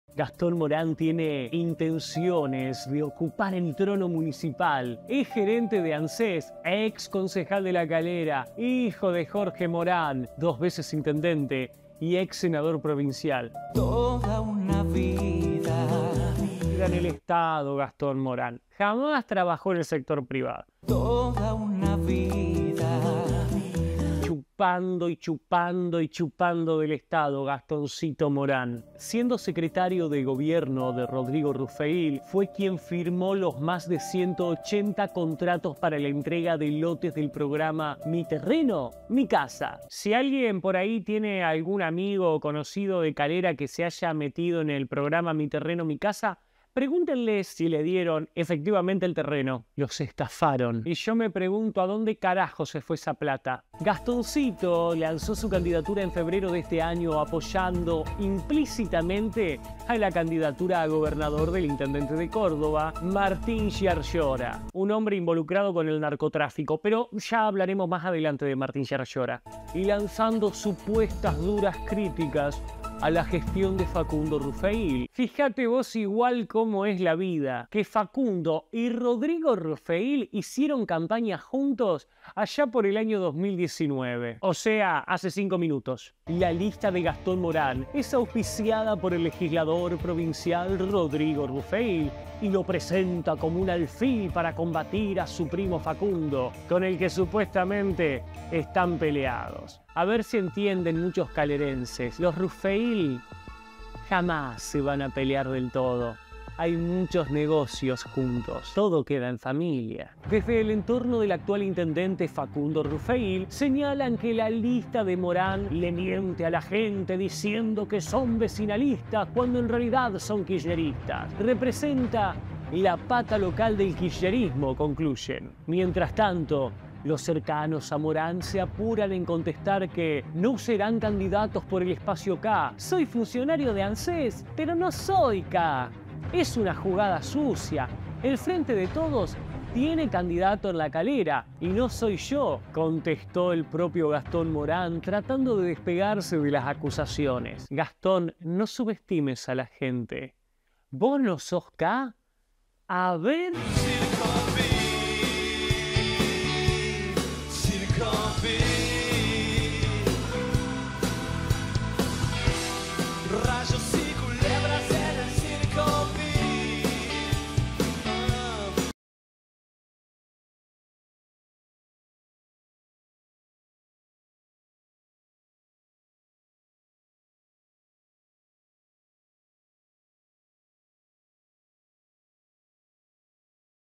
¡CUIDADO! | En Córdoba los K se están haciendo pasar por "OPOSITORES"
En este impactante video periodístico, el reconocido periodista El Presto destapa una sofisticada estrategia del kirchnerismo para mantenerse en el poder a toda costa. El objetivo es claro: evitar que las fuerzas opositoras logren la victoria en las próximas elecciones.

El Presto revela cómo el kirchnerismo ha recurrido a métodos astutos, como la infiltración de ex funcionarios ultra kirchneristas en partidos vecinalistas. Estos ex funcionarios son presentados al público como supuestos opositores, lo que genera una confusión en la ciudadanía y debilita la fuerza de las verdaderas alternativas opositoras.

La ciudad de La Calera se ha convertido en un claro ejemplo de esta maquinación política. Los primos Facundo y Rodrigo Rufeil han estado trabajando en complicidad con el kirchnerismo, colocando como candidato supuesto opositor a Gastón Moran. Este movimiento tiene como objetivo dividir aún más los votos opositores y debilitar las posibilidades de triunfo de las fuerzas que realmente representan un cambio.

En este revelador reportaje, El Presto analiza en detalle las conexiones entre los ex funcionarios ultra kirchneristas, los partidos vecinalistas y los candidatos supuestos opositores. A través de pruebas contundentes y testimonios de fuentes confiables, se expone la trama urdida por el kirchnerismo para mantener su control sobre el poder.

Este video es un llamado a la ciudadanía a estar alerta y no dejarse engañar por las tácticas manipuladoras del kirchnerismo. Es fundamental estar informados y tomar decisiones conscientes en el próximo proceso electoral, para evitar que nuestras voces sean silenciadas y nuestras opciones sean limitadas.